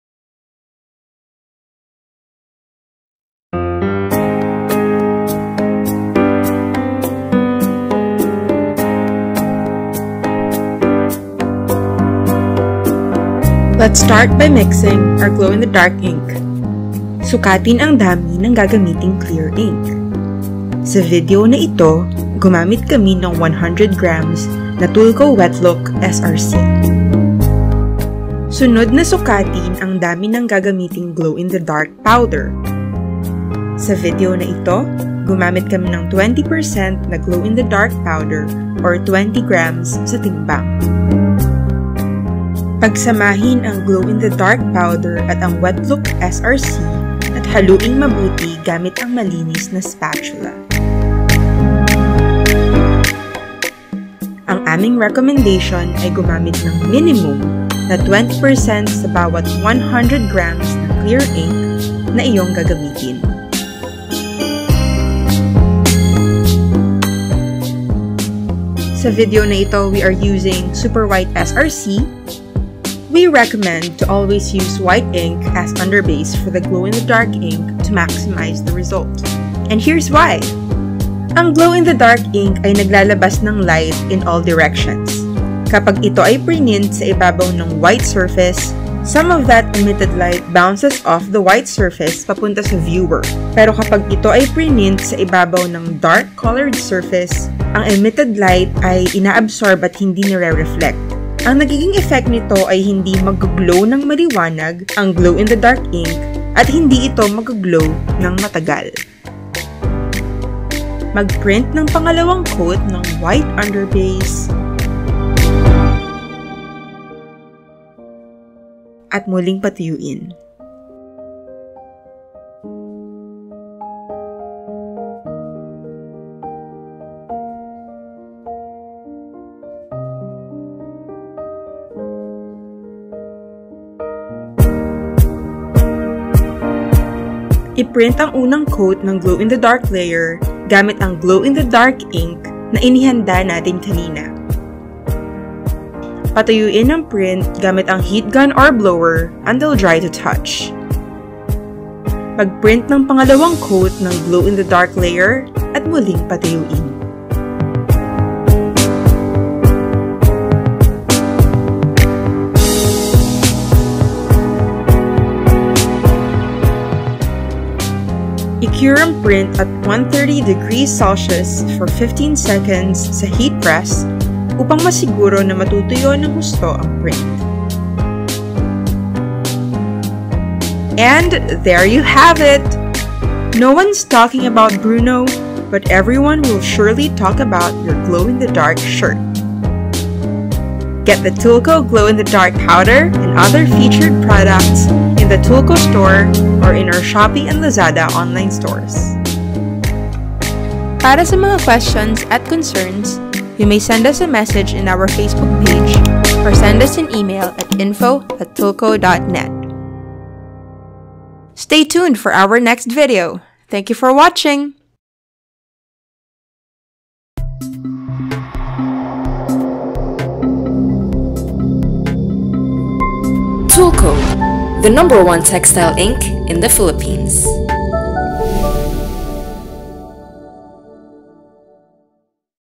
Tulco Glow in the Dark Powder + S-RC Series
How do you actually make a t-shirt print glow? Watch to find out!
Here's a tutorial featuring the Tulco Glow in the Dark Powder + Wetlook S-RC!

Recommended Formula:
Minimum of 20% Glow in the Dark Powder for every 100g of Clear Ink.

The Glow in the Dark Powder consists of very fine crystals of zinc sulfide doped with copper (ZnS:Cu) and it does not contain any radioactive substance

The Tulco Glow in the Dark Powder is available in the Tulco Store, Shopee, and Lazada!